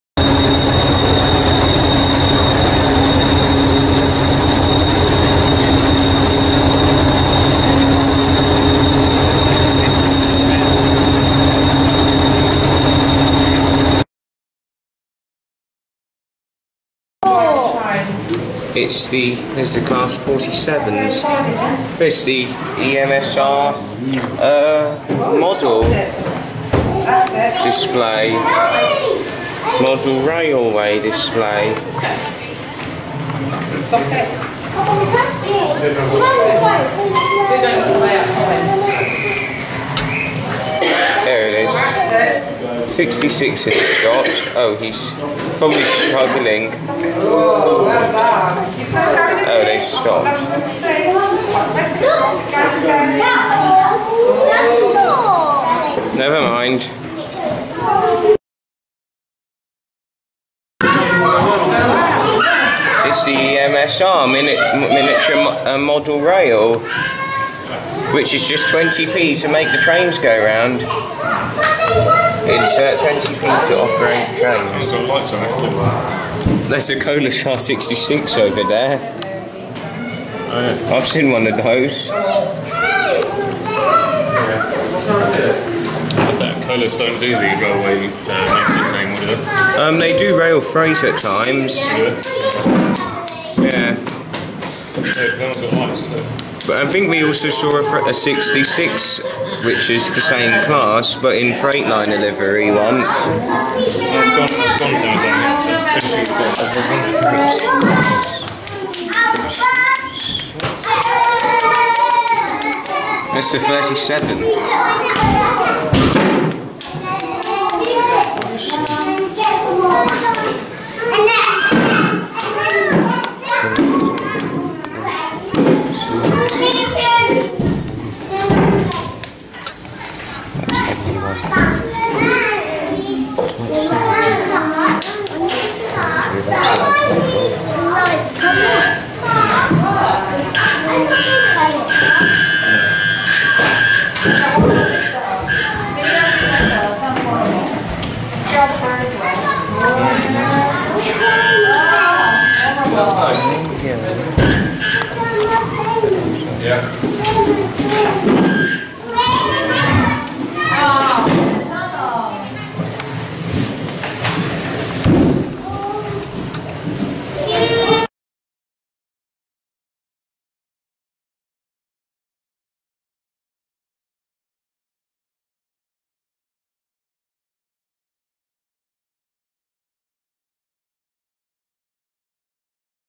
EMSR 25/06/2017 - EMSR Model Railway
Hello there guys this is me "Foxy313" and yes I went to the Eastbourne Miniature Steam Railway (EMSR) on a chilly Sunday 25th June 2017 along with my Dad and we stupidly enjoyed ourselves largely :-). Eastbourne Miniature Steam Railway is a lovely little railway and I really like it's route :-). But yes in this video it features me "Foxy" filming the EMSR Hornby model railway which is what the EMSR practically has :-). Also, please note that this EMSR video is the last of all the EMSR videos just to let you guys know :-). Enjoy the video and if anybody is facinated to find out about the open days for the Eastbourne Miniature Steam Railway (EMSR) the link will be found straight under here for only those who are interested.
Stay awesome everybody, plus always enjoy yourselves :-D :-D.
From your friend Foxy :-).